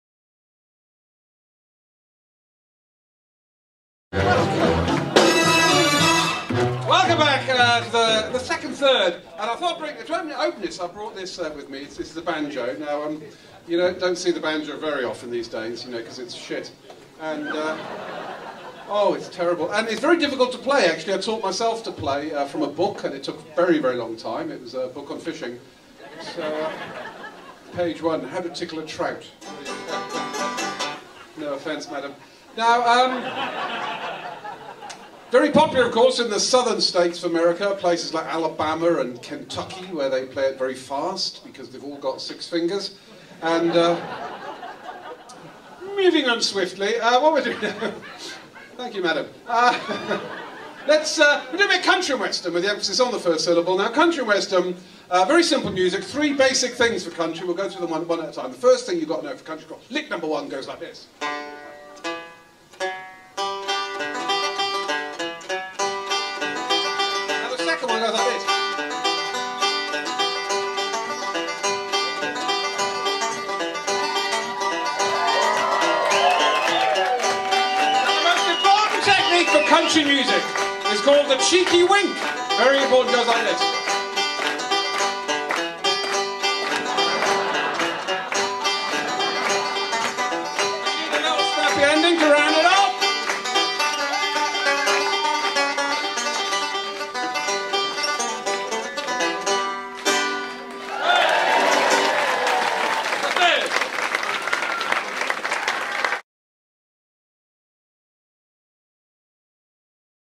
Simon Fox at the Glee Club
Comedian Simon Fox plays some country banjo at the Glee Club Birmingham